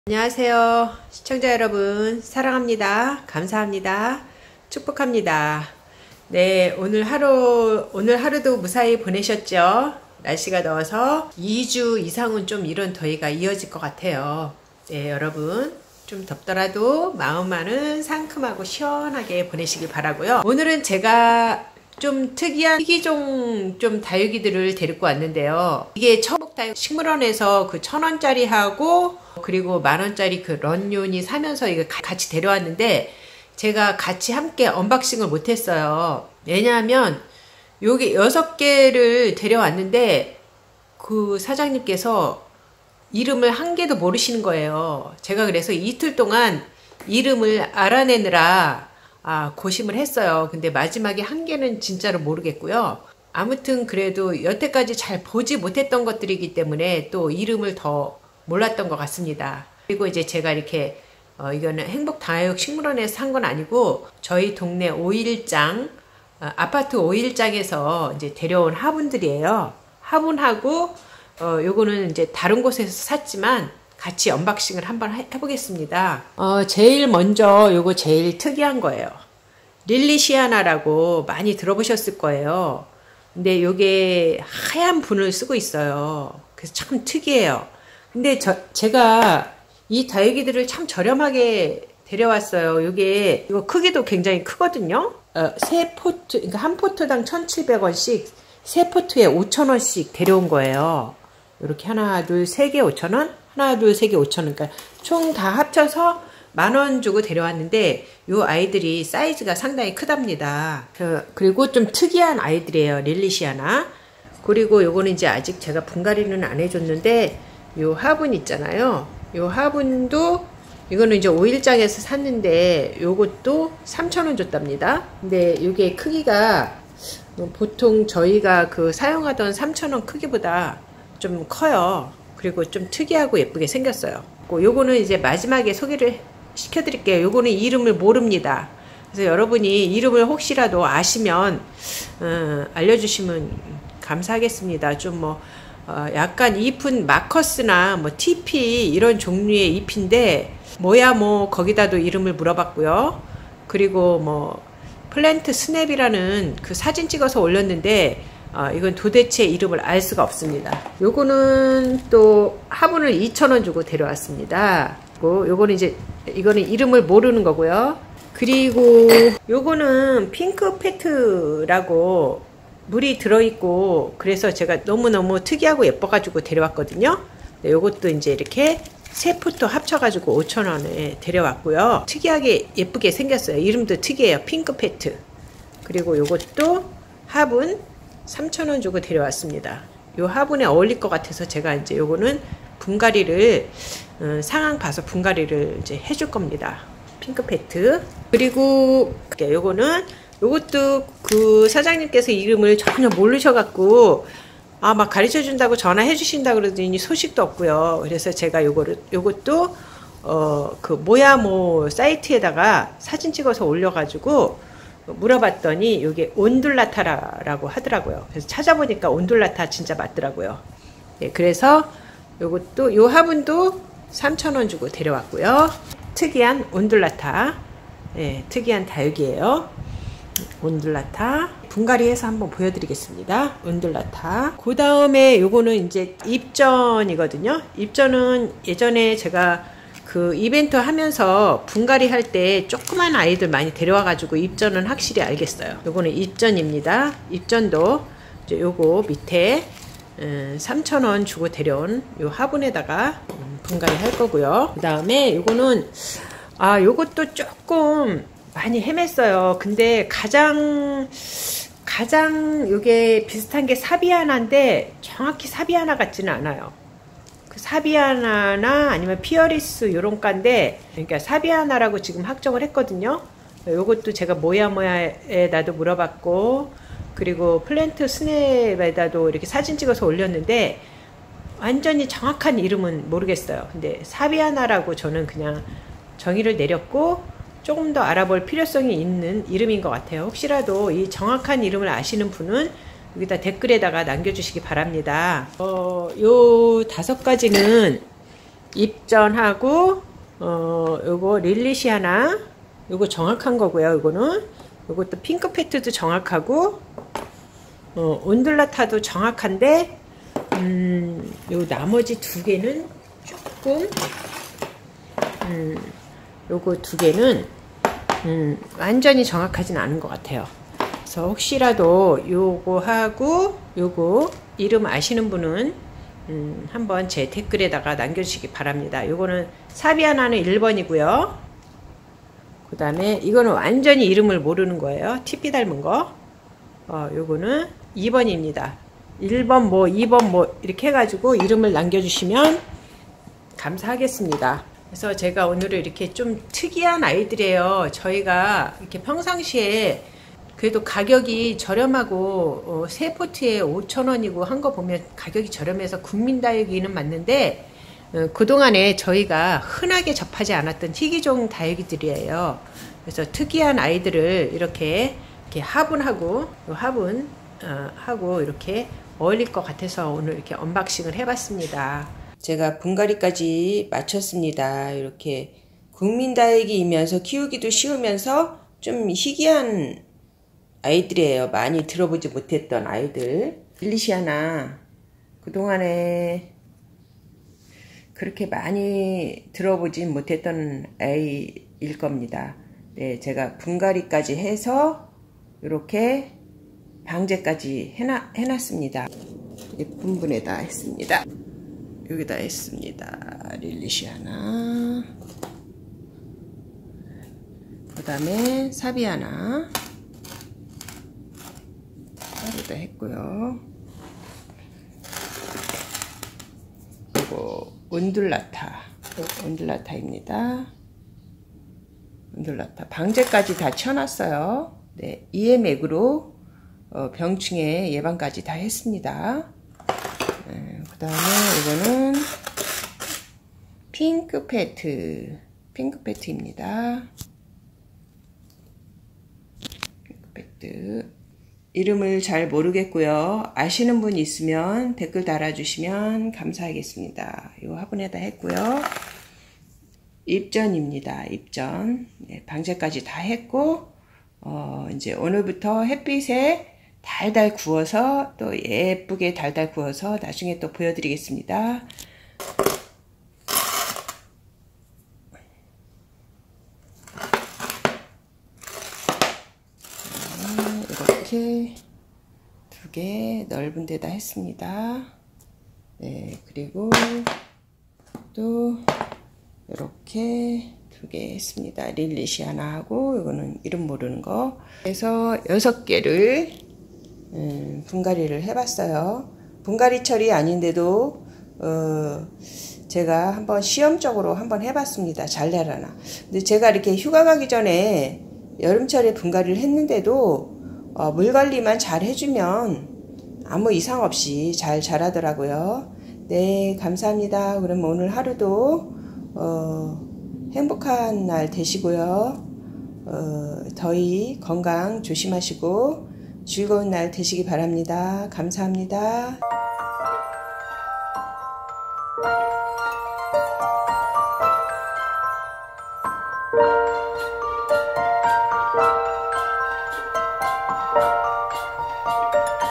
초보자분도 키우기쉬운 보기드문 희귀종 국민다육 천후엽변경 릴리시아나,온둘라타,사비아나,입전등 Succulents
안녕하세요~!!
제가 10시쯤 올링영상이
끊김현상이 있어서 다시 재업로드 했습니다
이미 조회 하신분들께
죄송하구요
그리고 핑크페트로 올렸던 다육이름
은 천후엽변경 으로 정정해드립니다

오늘도 더위조심하시고
건강 잘챙기시며 시원하게
보내시길 바랍니다
늘 항상 감사합니다
              사랑합니다
              축복합니다